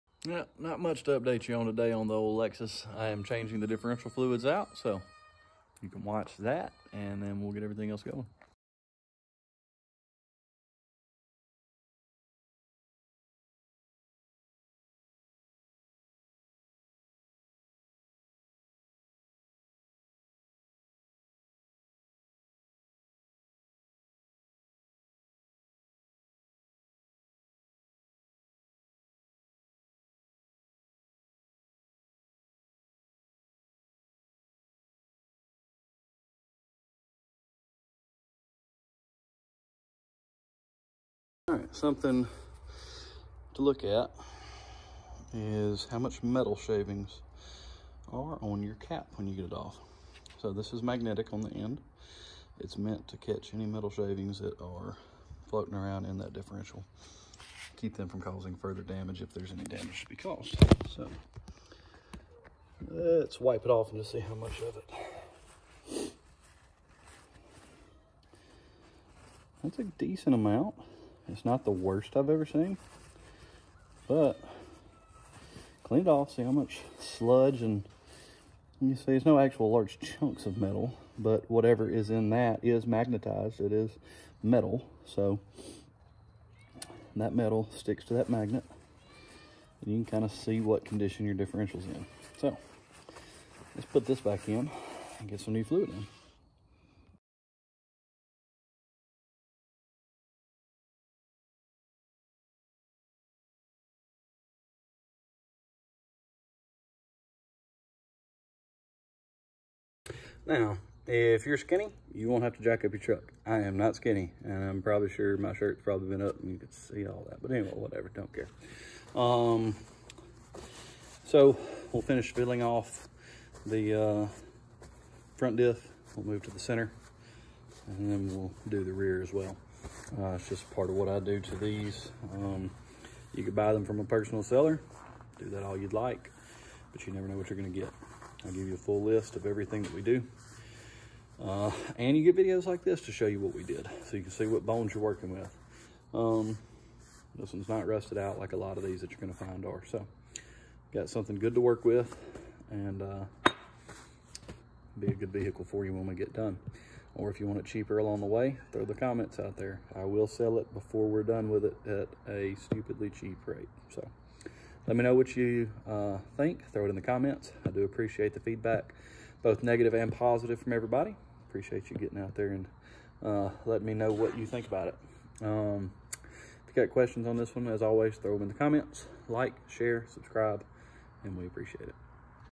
2006 Lexus GX 470 front differential fluid change project Black Beauty
Looking at our 2006 project Lexus getting the differentials all swapped out to new fluid.